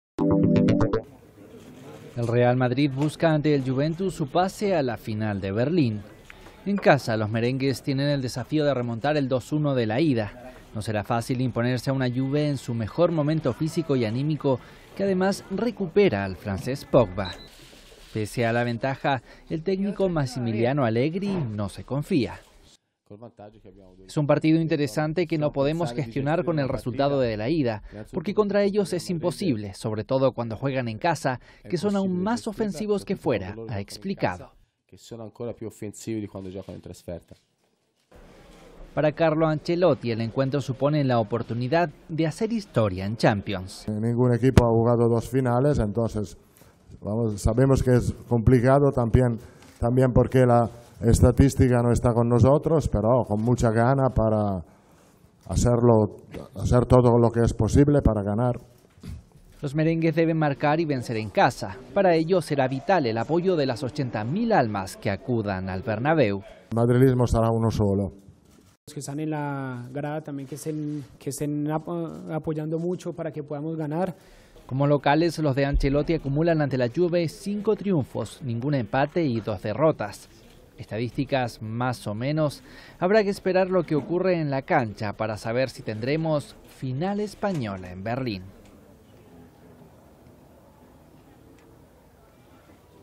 Real Madrid-Juventus, duelo por el pase a la final de Champions
Madrid, 12 may (EFE).- El Real Madrid busca una remontada europea que hace años no consigue, alejado de aquellas noches míticas que dieron forma a su leyenda, en un pulso por la presencia en la gran final de Berlín con un Juventus en mejor momento físico y anímico tras el 2-1 de la ida, que además recupera al francés Paul Pogba.